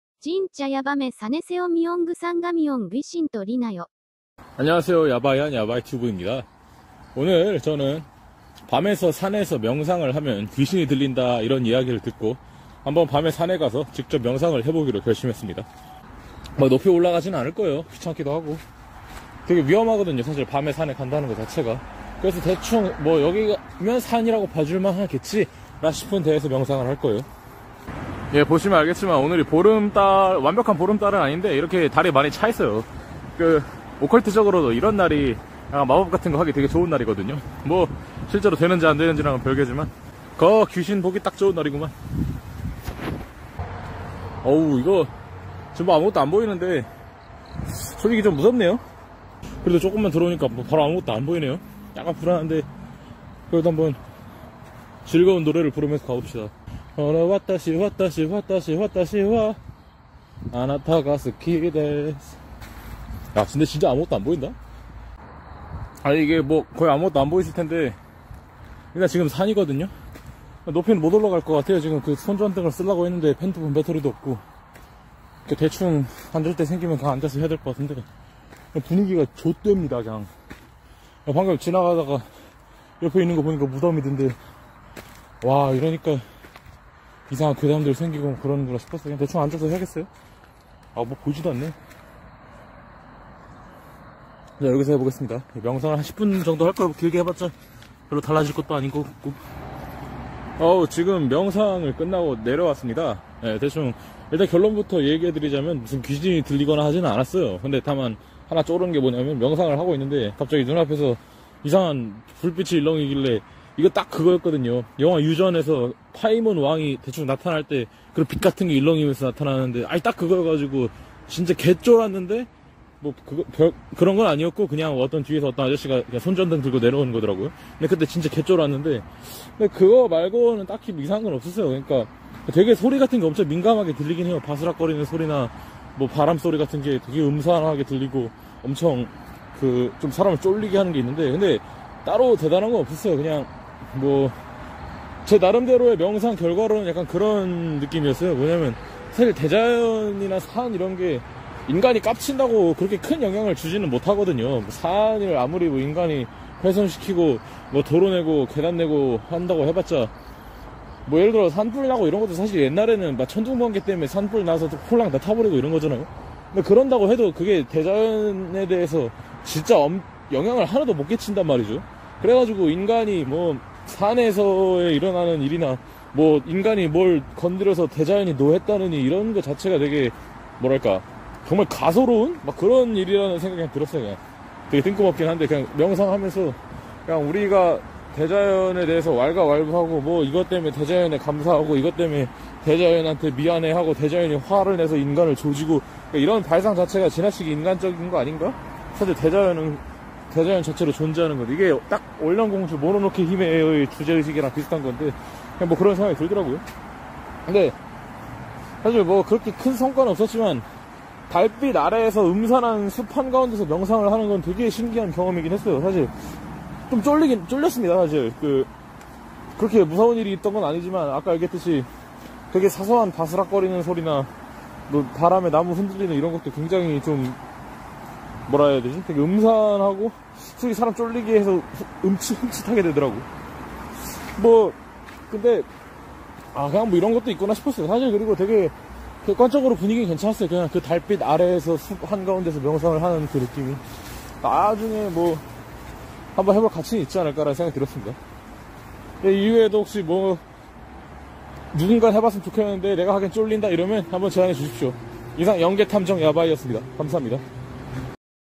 밤에 산에서 명상하면 귀신들린다? 직접 해봄 [YABAITUBE]
영계탐정 yabai